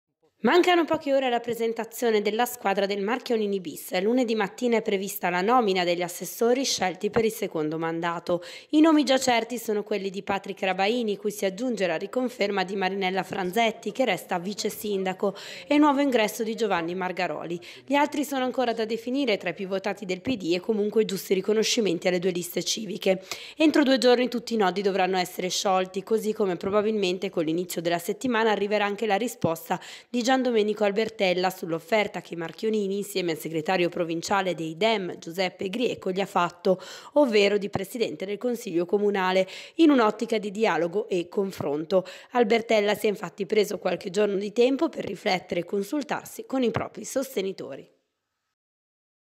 Lunedì pomeriggio Marchionini presenta la Giunta
15 giugno 2019 - Per il momento assessorati certi per Rabaini, Franzetti e Margaroli